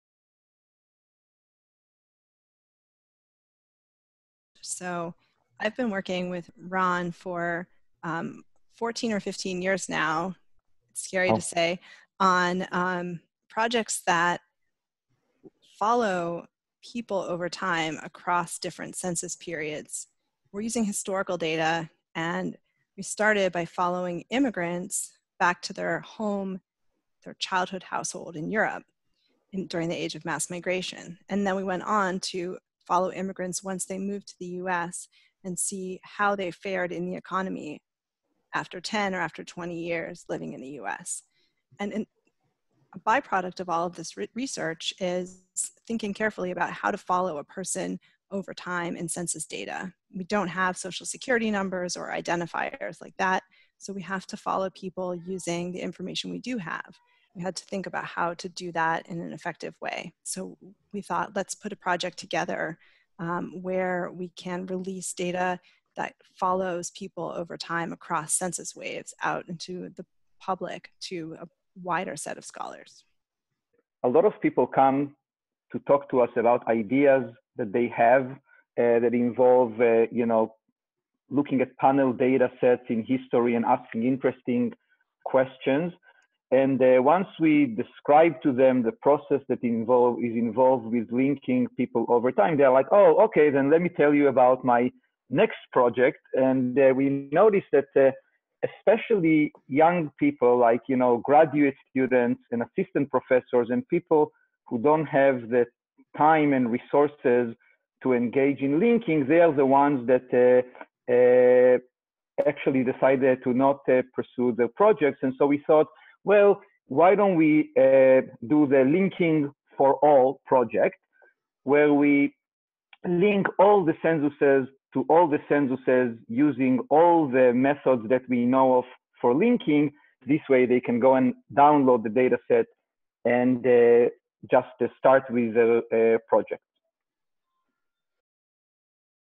The Origins of the Census Linking Project
Princeton Professor Leah Boustan and Stanford Professor Ran Abramitzky discuss the origins of the Census Linking Project, which offers researchers the ability to create longitudinal datasets using historical US Census data (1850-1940).